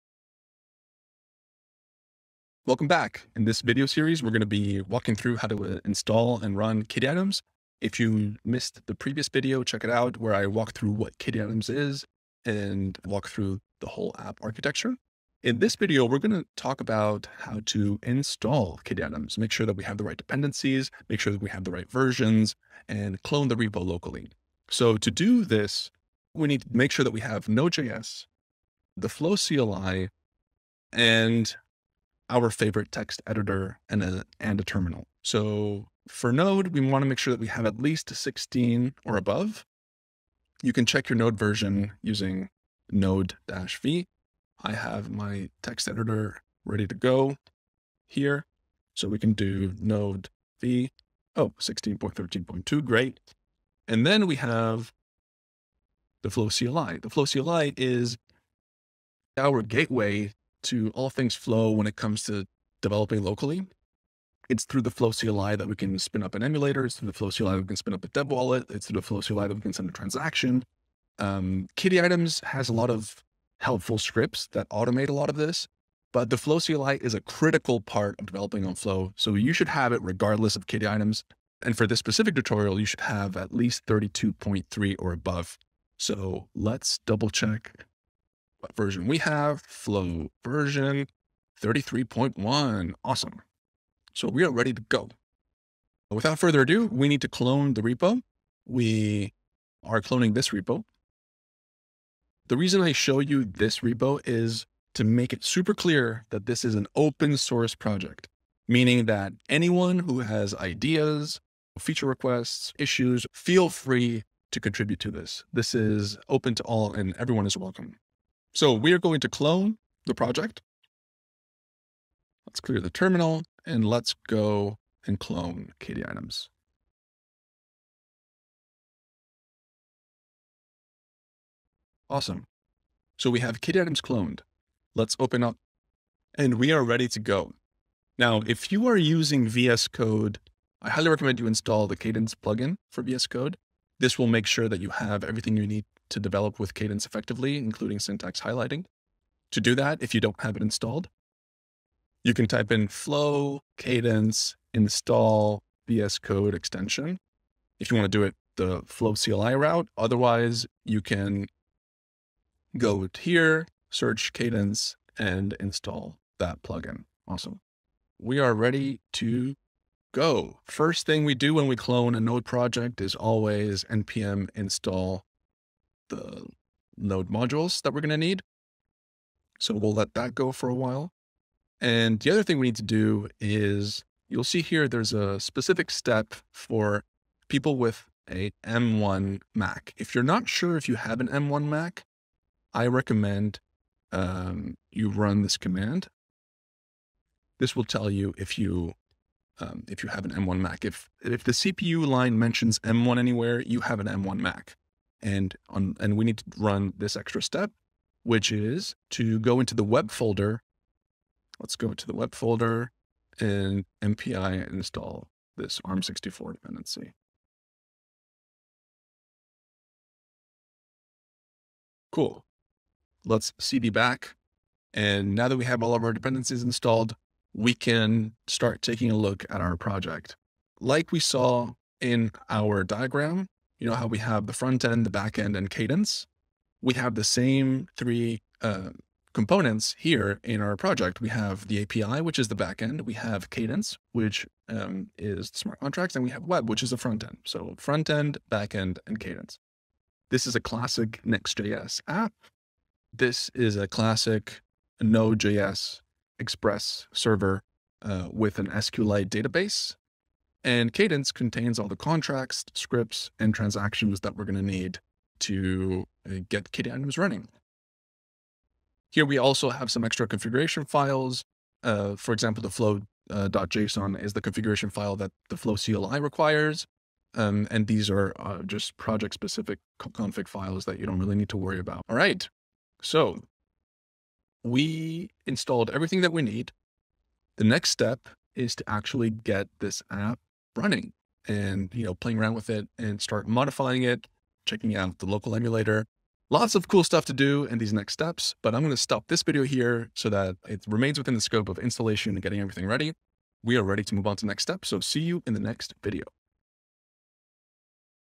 Kitty Items Walkthrough - Part 2
This video series walks you through setting up and running Kitty Items, our complete NFT marketplace inspired by CryptoKitties, a web3 game that put NFTs on the map. You can use it as a blueprint for your own marketplace and learn how to develop on Flow.

Ask questions in the Discord
Reach out to Andrea on Twitter